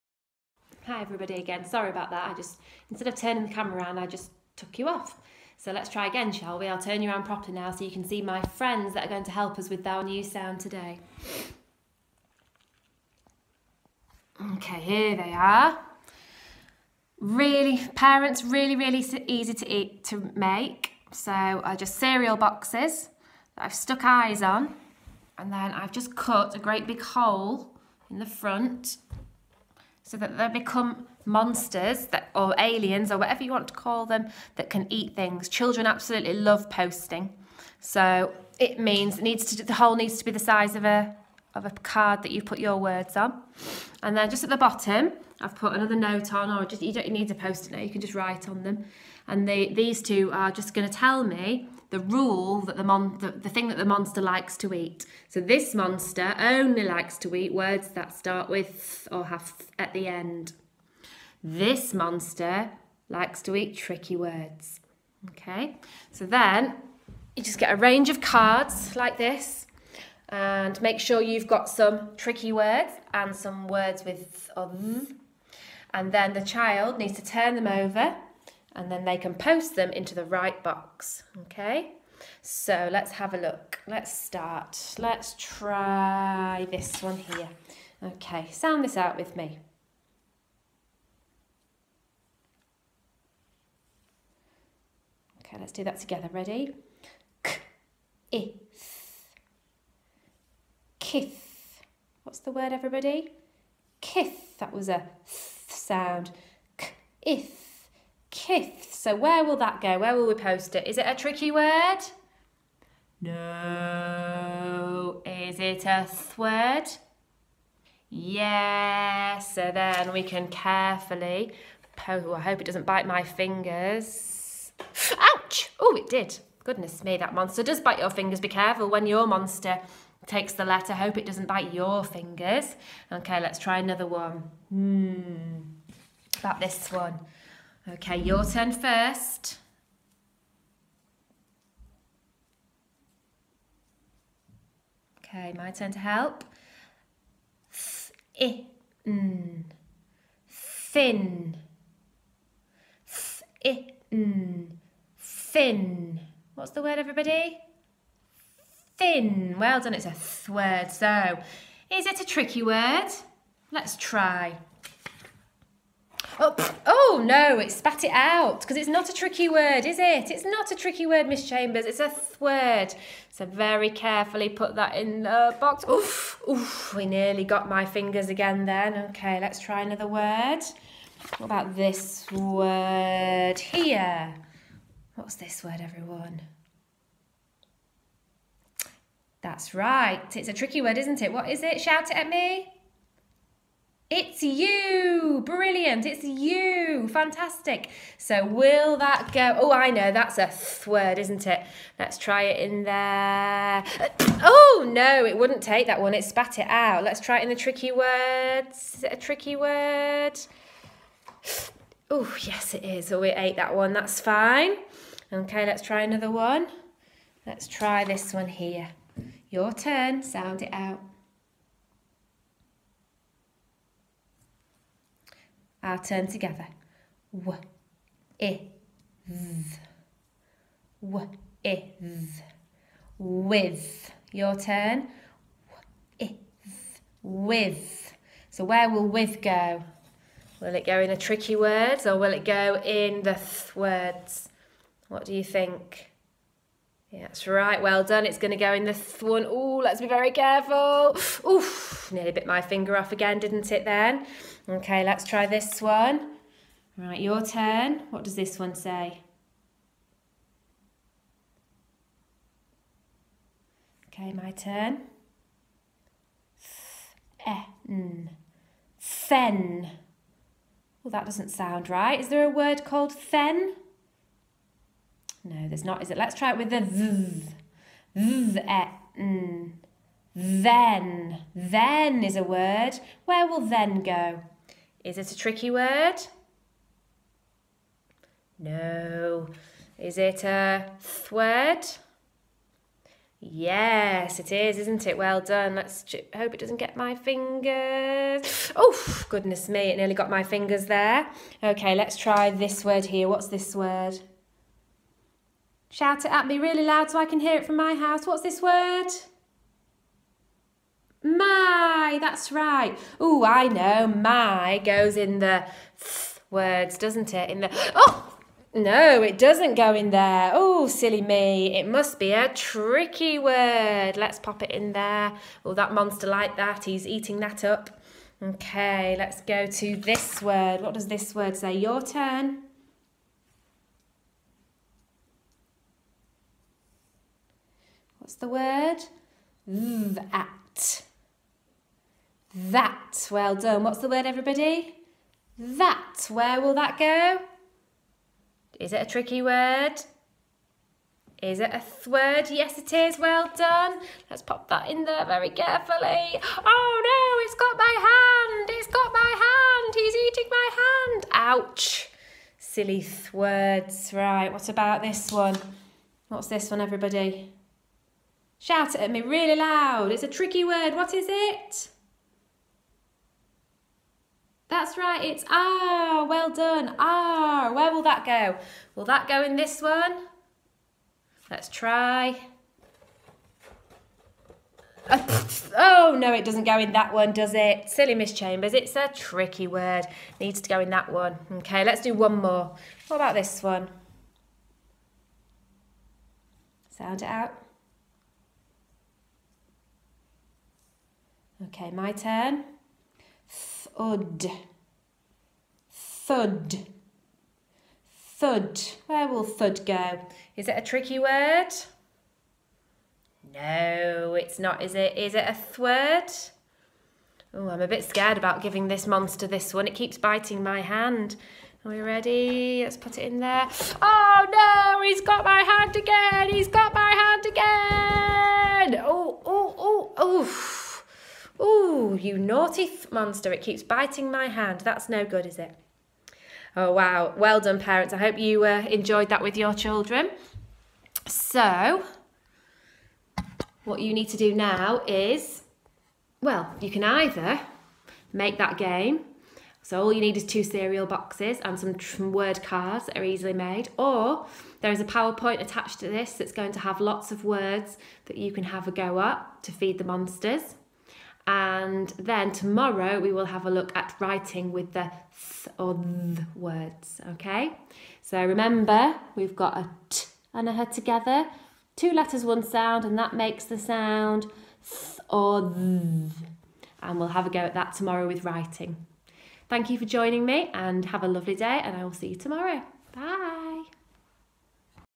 Phonics 11th may reading with Th (part 2)
This video is part 2 of teaching  you how to read with the sound th